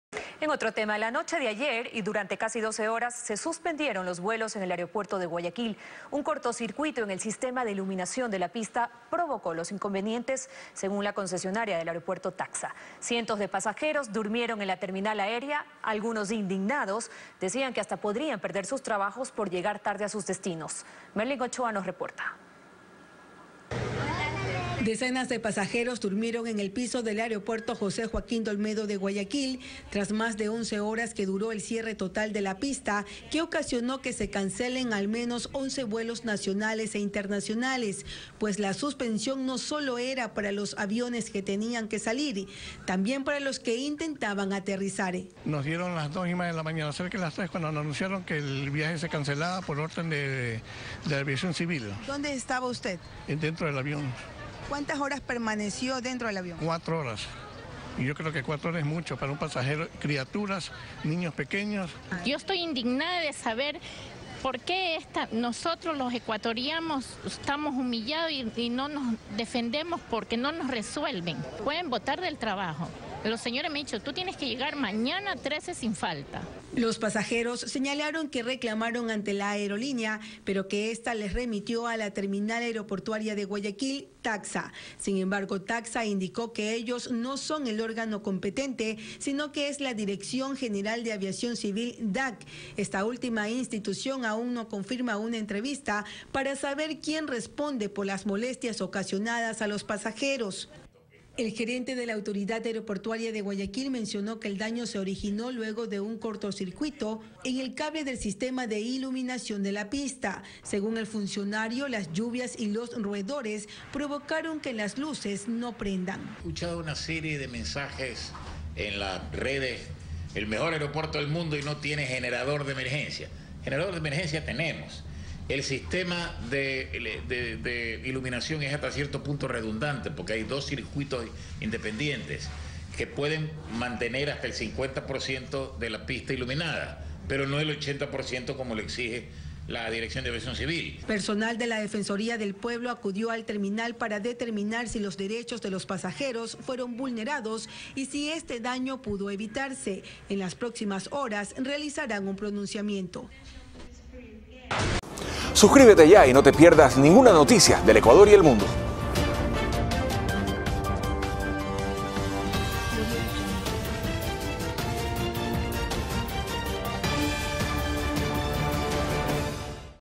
Por qué se registró cierre de operaciones en el aeropuerto José Joaquín de Olmedo de Guayaquil
DALE CLIC EN “ME GUSTA” - COMPÁRTELO